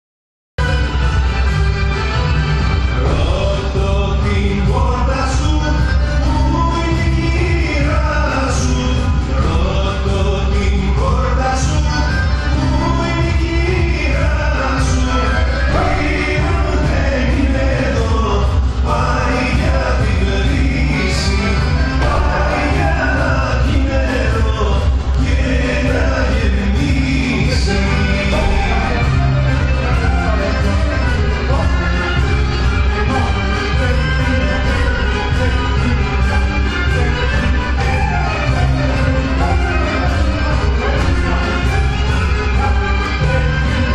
Kalamatiano Balkanci
Каламатяно в изпълнение на групата по гръцки танци за начинаещи на школа по български и гръцки фолклорни танци Балканци.
Занятията се провеждат във вторник и четвъртък от 19,30 до 20,30 ч. в Зала 1 на Дондуков 11, ет. 2.
Все още не е късно да се включите, ако ви харесва!